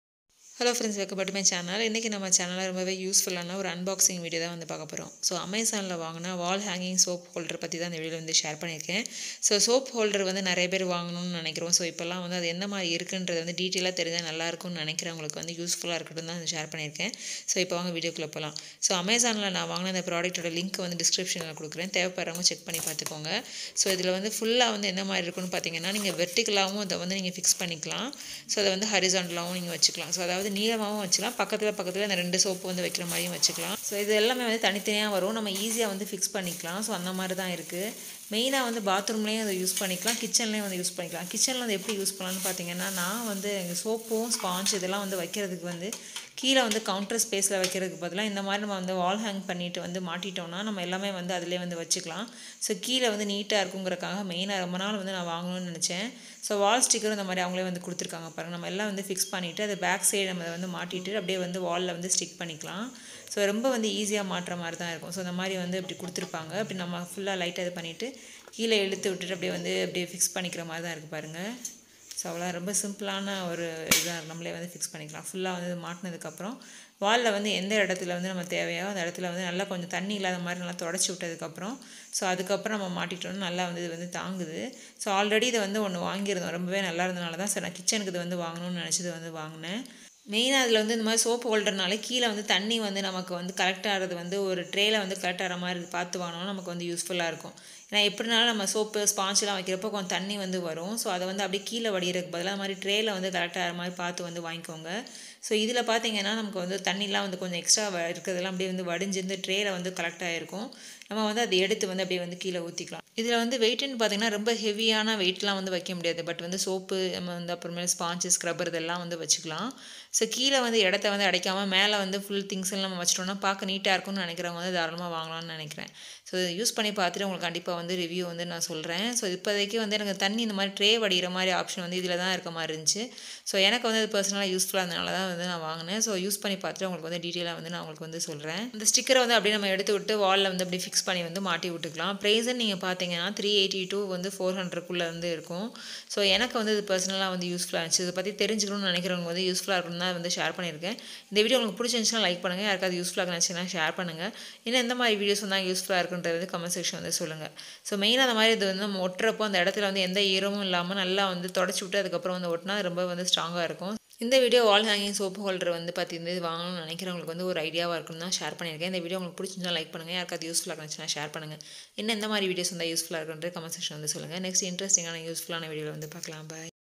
Amazon shopping haul/wall hanging soap holder/shopping haul in Tamil
Dear friends
In this video I am sharing useful shopping haul if you know about the wall hanging soap holder this video will be helpful so watch this video for upcoming useful videos subscribe to my channel.